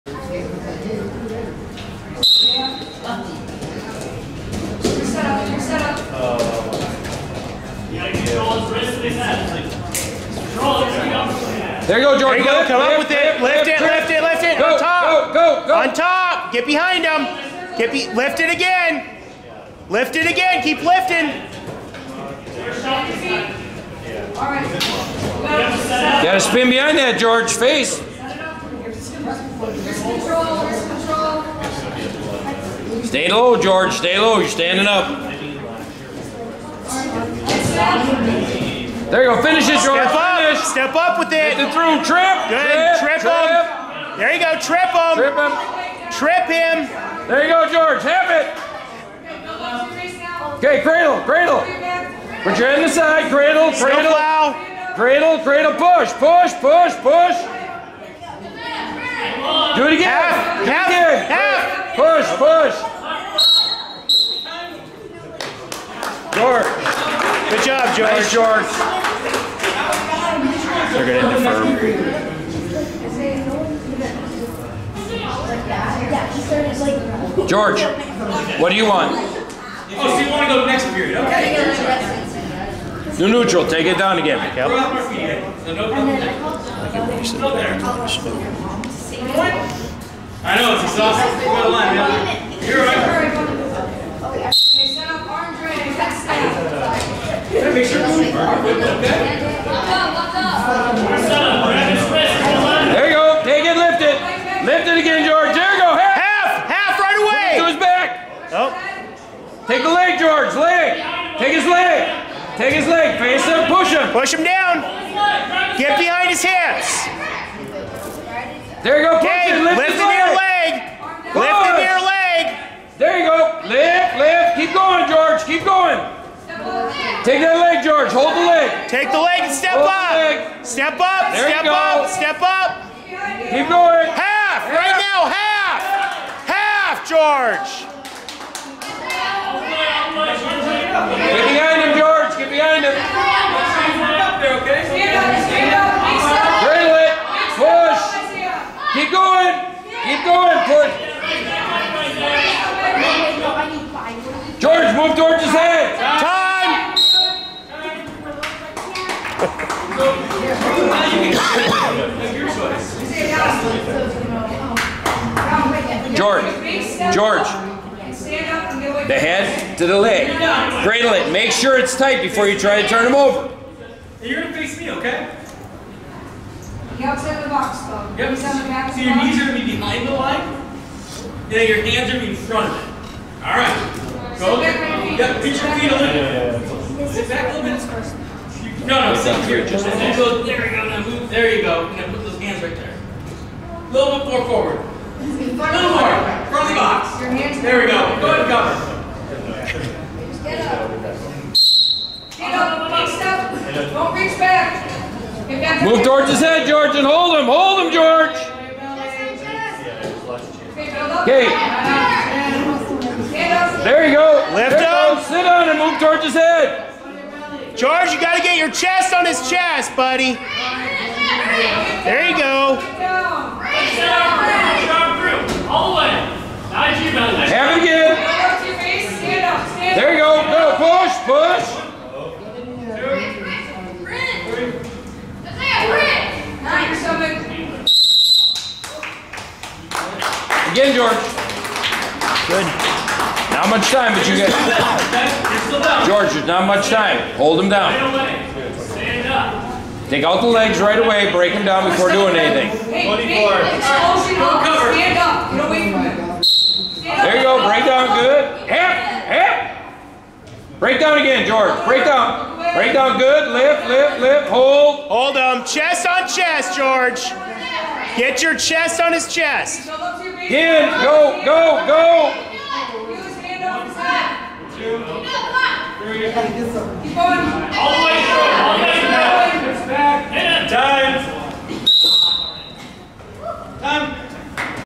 George 2 vs Whitman Middle School – Longfellow Wrestling Match
Longfellow Middle School vs Whitman Dual Wrestling Meet 2022/23 Season

His second match of the night